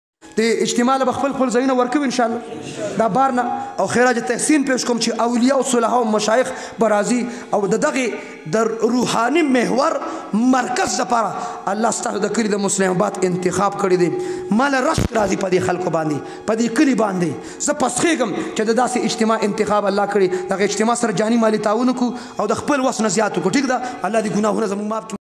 MUFTI SARDAR ALI HAQQANI SAB NEW WADA FUNNY BAYAN | MWJ STUDIO | MAULANA Peer Hizb Ullah JAN SAHIB
مفتی سردار علی حقانی صاحب بیان
9. 10 دسمبر 2021 باندے پہ خانقاہ فریدیہ ختم نبوت ترناب ضلع چارسدہ کے یو عظیم الشان اجتماع کیدونکے دہ
ٹول پکے شرکت ضرور اوکے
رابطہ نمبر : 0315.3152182